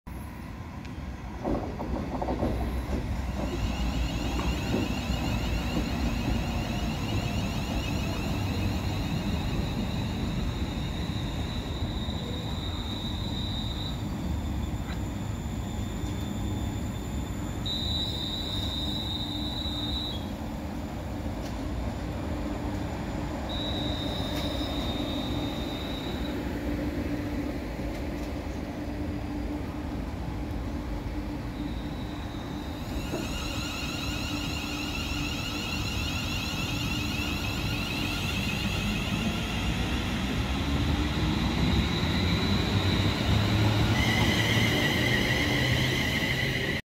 EMR Class 360 - 360016 passing Harpenden Station on 17/04/24
EMR Class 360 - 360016 passing Harpenden Station on Slow Lines  on 17/04/24